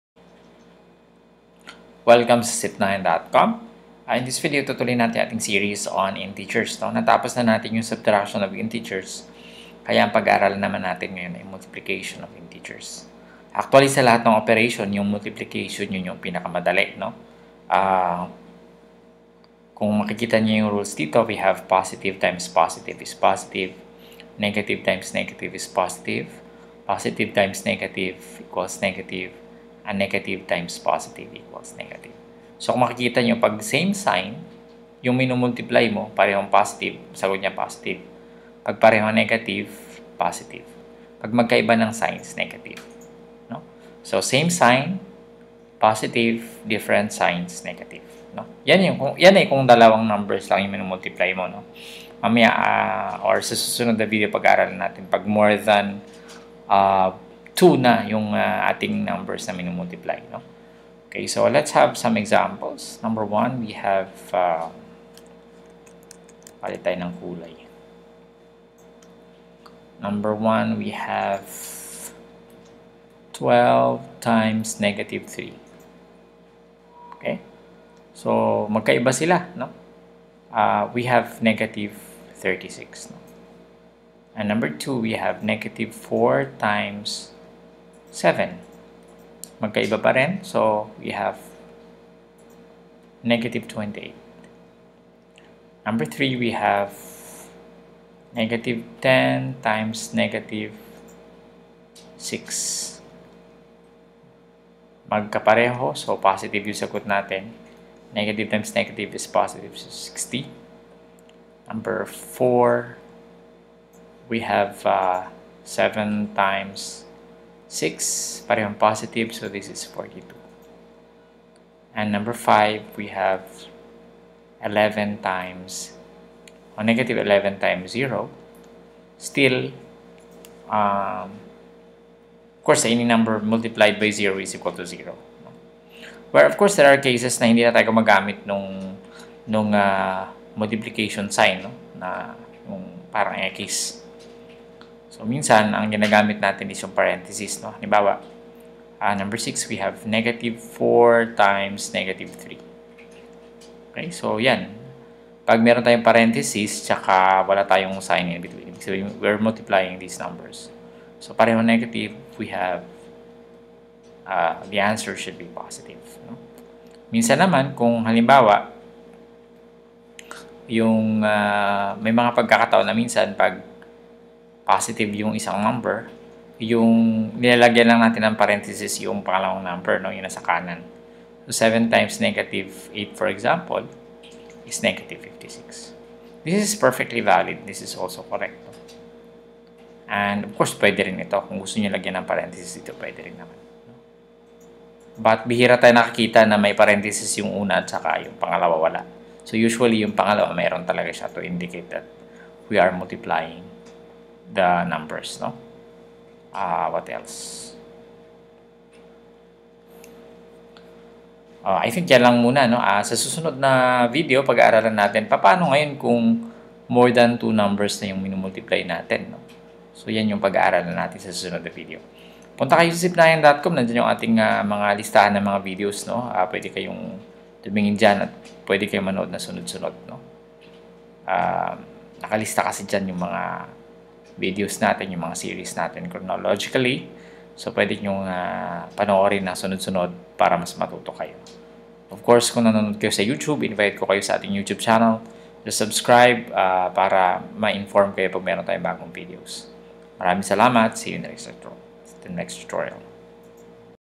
MI1 How to Multiply Integers Part 1
In this video, we are going to learn how to multiply integers.